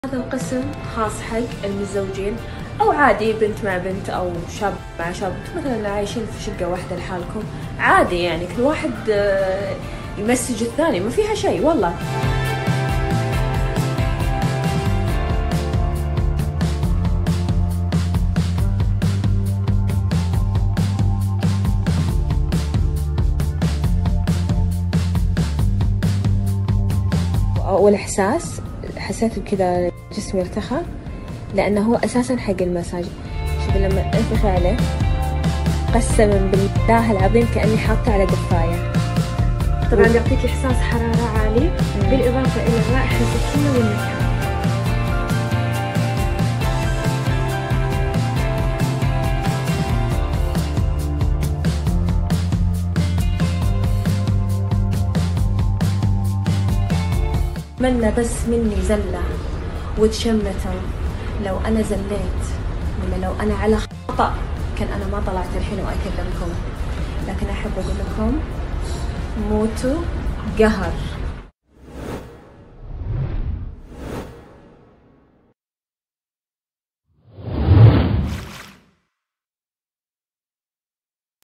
Une huile de massage fait polémique en Arabie Saoudite !
Cette Saoudienne fait la promotion d’une huile de massage pour exciter la libido chez les hétérosexuels et les homosexuels !